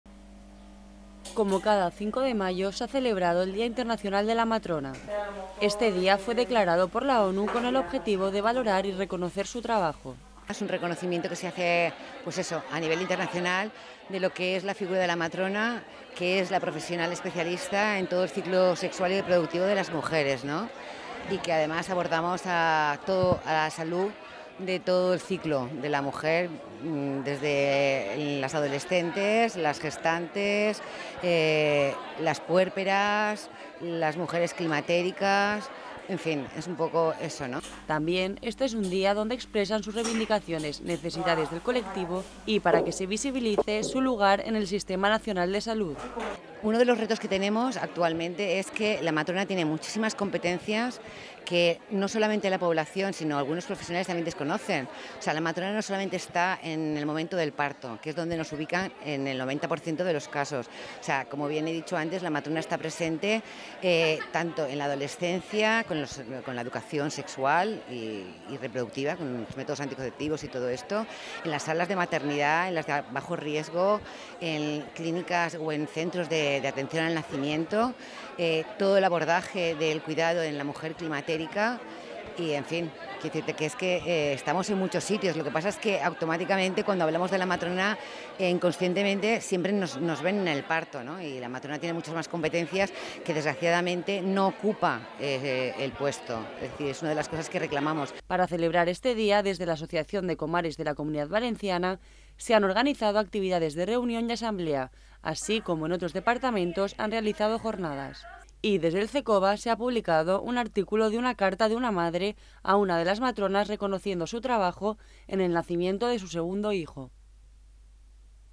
Día Internacional de la Matrona | Cecova TV
Como cada 5 de mayo, se ha celebrado el Día Internacional de la Matrona. Este día fue declarado por la ONU con el objetivo de valorar y reconocer su trabajo.